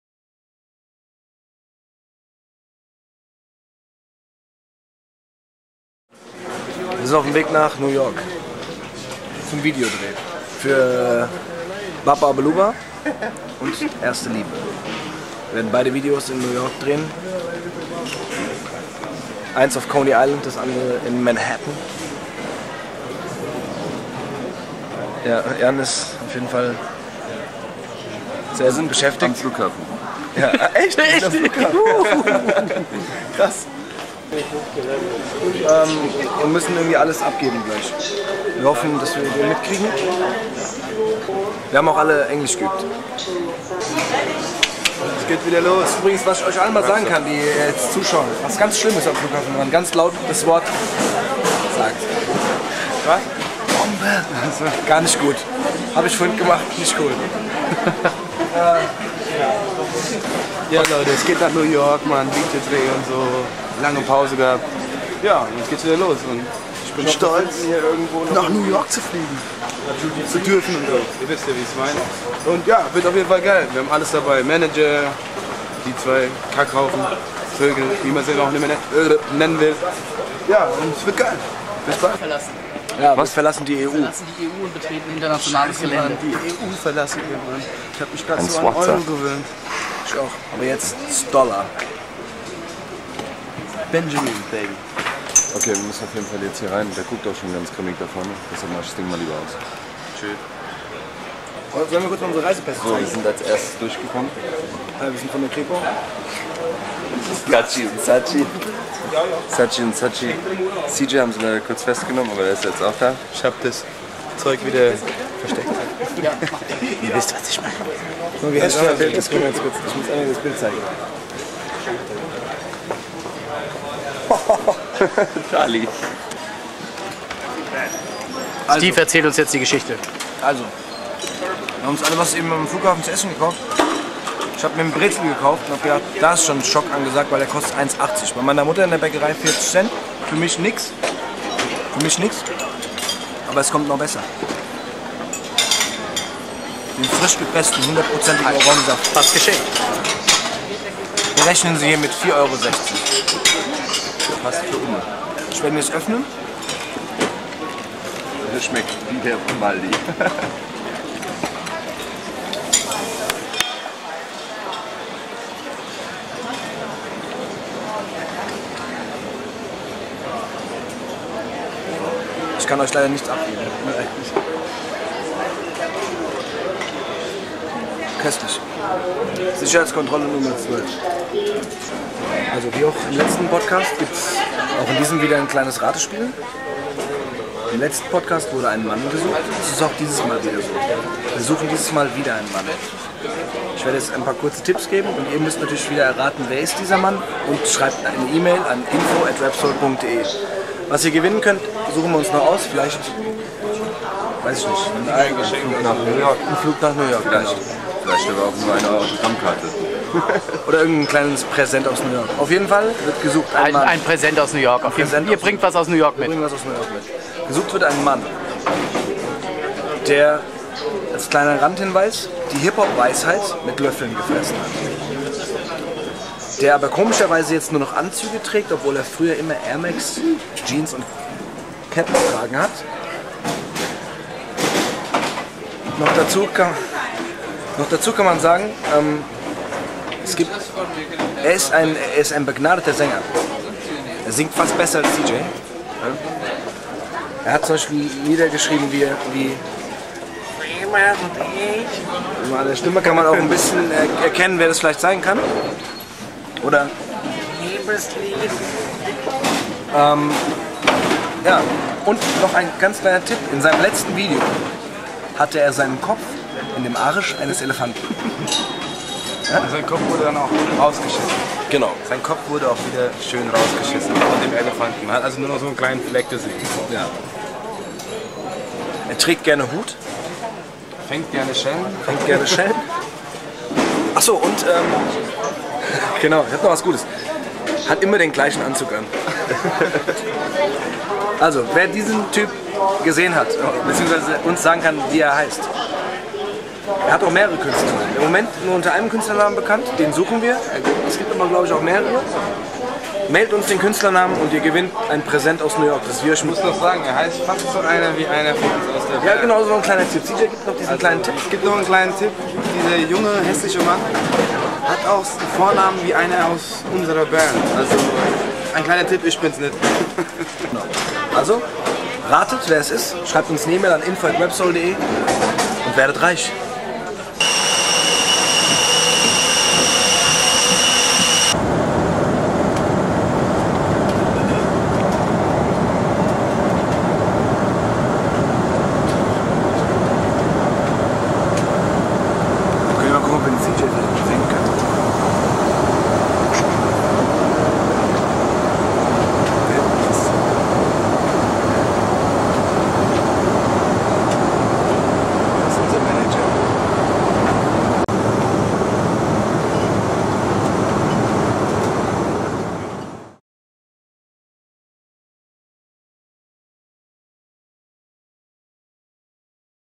RAPSOUL Podcast 23 / Auf dem Weg nach NYC
RAPSOUL fliegen zum Videodreh nach New York. Wie schon in der COMET-Folge entschließen Steve, CJ und Jan sich spontan, ein kleines Gewinnspiel zu veranstalten, also haut rein und macht mit!